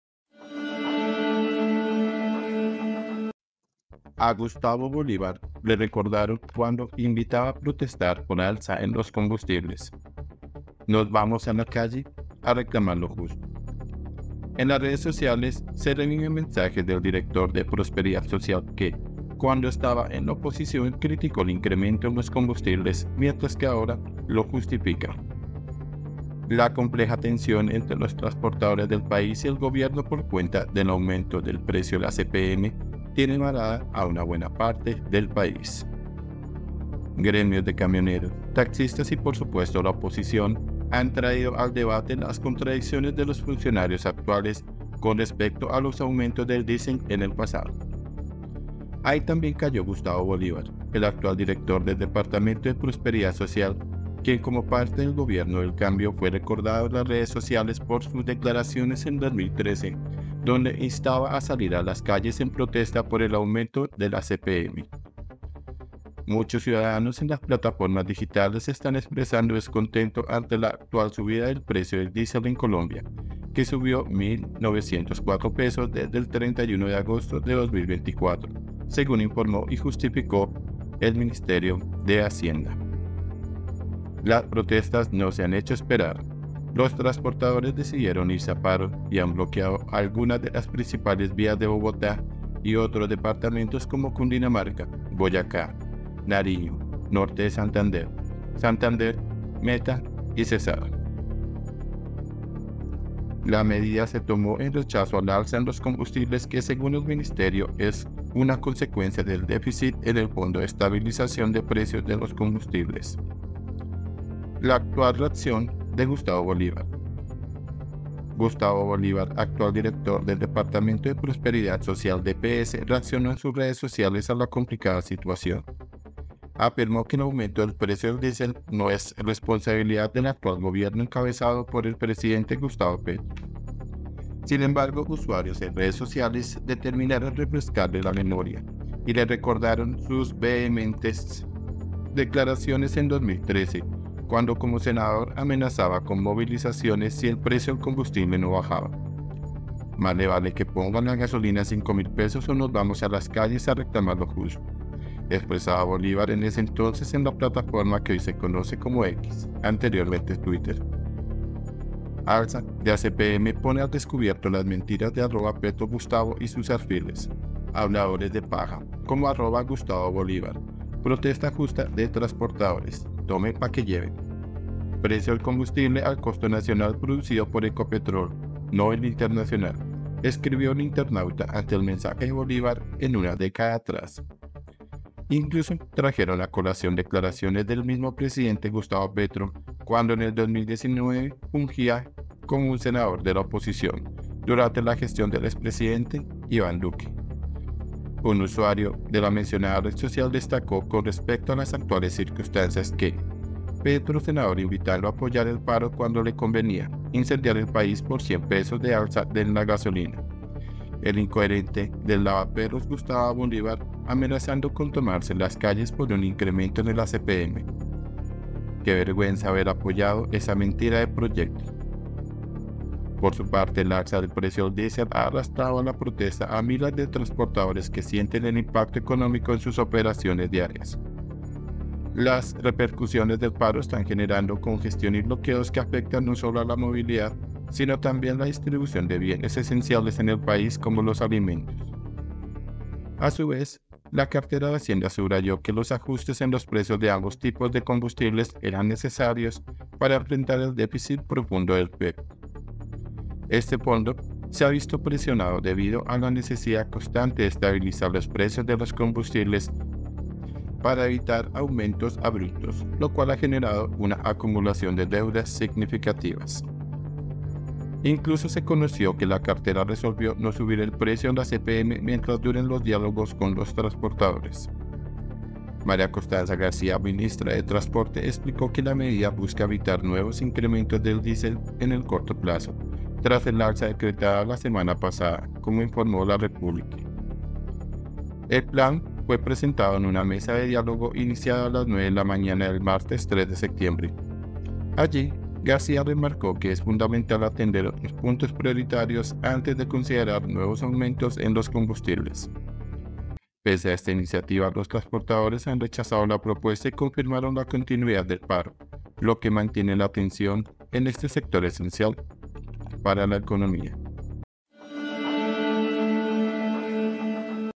INCOHERENCIA DE GUSTAVO BOLIVAR ❌ LE RECUERDAN MENSAJES EN LA RED SOCIAL X
A Gustavo Bolívar le recordaron cuando invitaba a protestar por alza en los combustibles: “Nos vamos a la calle a reclamar lo justo”
INFOBAE